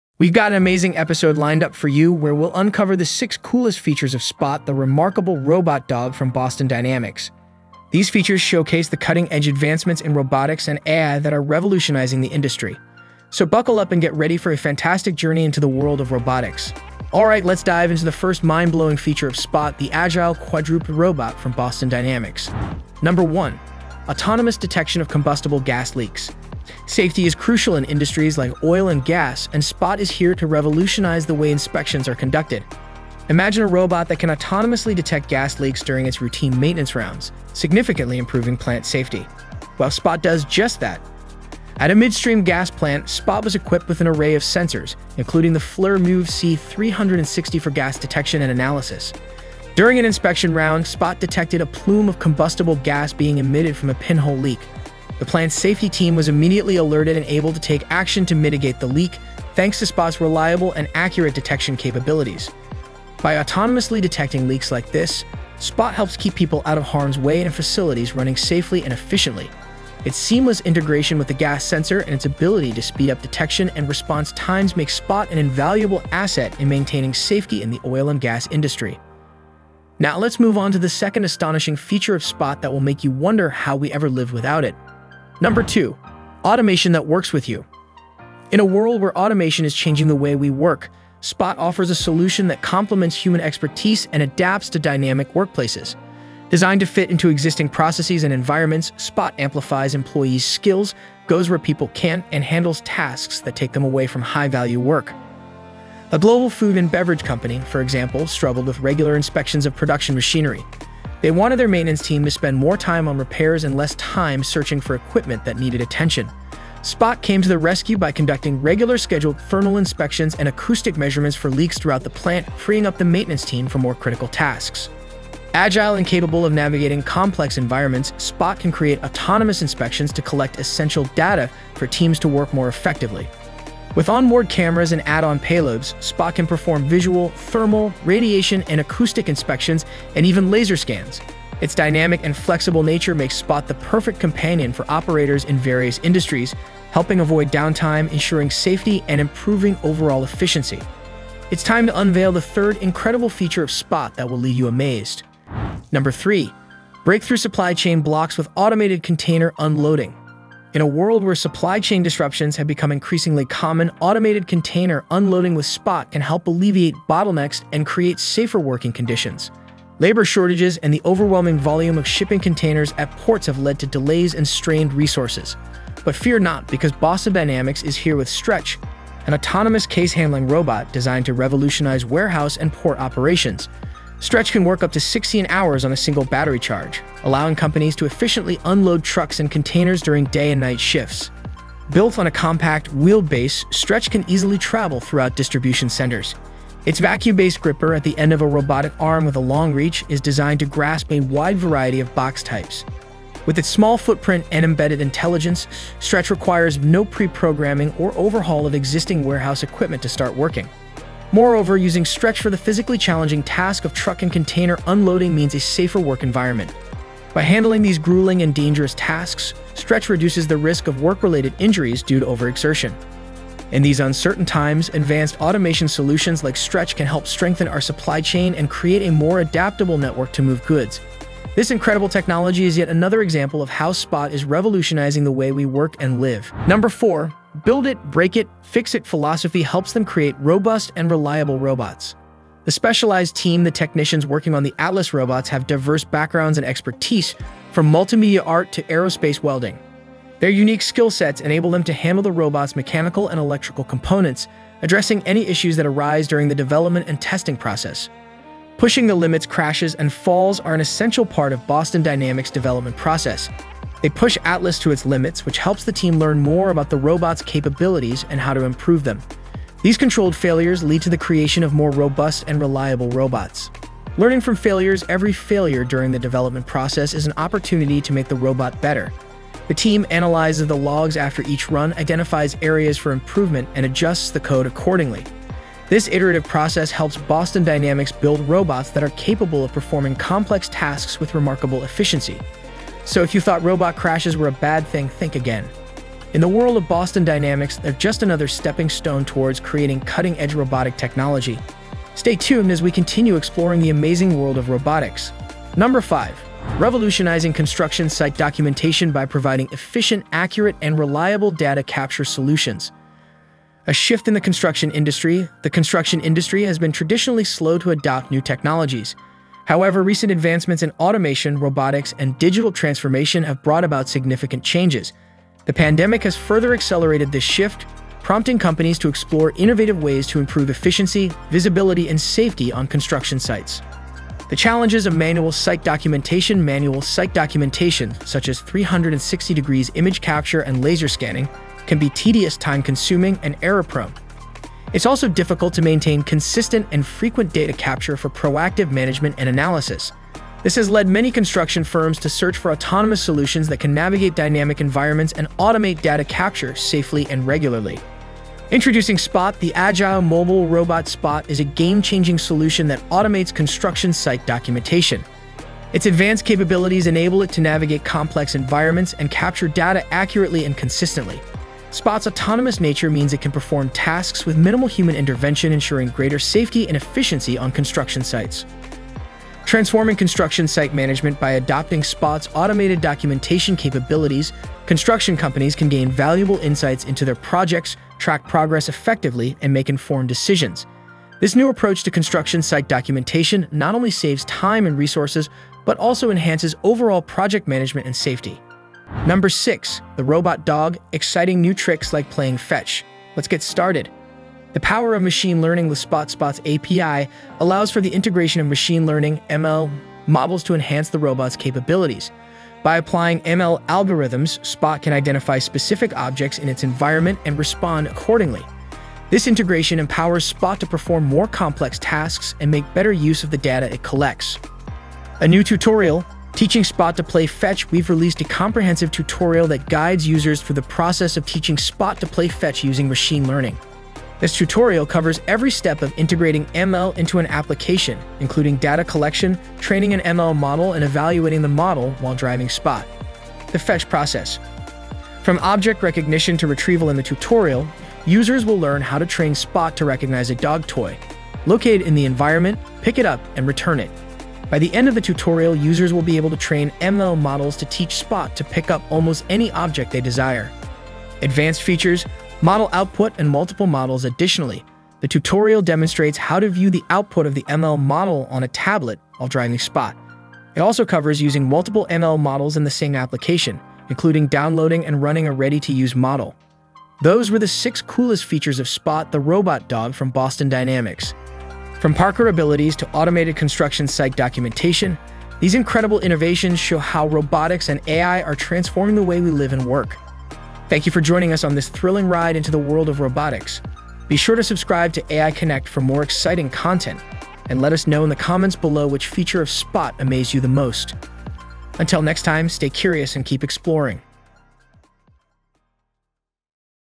Spot Unleashed 6 Mind-Blowing Features of Boston Dynamics' Robot Dog
🤖🐶 Welcome to our latest video, "Spot Unleashed - 6 Mind-Blowing Features of Boston Dynamics' Robot Dog!" In this thrilling journey into the future of robotics, we delve into the groundbreaking technology that makes Boston Dynamics' Spot not just any robot dog, but a pioneering mechanical marvel.

In this video, we will:

1️⃣ Autonomous Detection of Combustible Gas Leaks
2️⃣ Automation that Works with You
3️⃣ Automated Container Unloading
4️⃣ 'Build It, Break It, Fix It'
5️⃣ Construction site documentation
6️⃣ New tricks for your pet

From navigating difficult terrains to performing complex tasks, Spot is not just a robot; it's a revelation in the world of robotics. Whether you're a tech enthusiast, an engineer, a student, or simply curious about the future of automation, this video is a must-watch!

💡 Did you know? Boston Dynamics has been pushing the boundaries of robotic design, bringing science fiction to reality. Spot, their most interactive and advanced creation to date, is leading the way in demonstrating the potential applications and benefits of robotics in our everyday lives.

Have questions or thoughts on Spot? Leave them in the COMMENTS section below! We'd love to hear from you and keep the conversation going!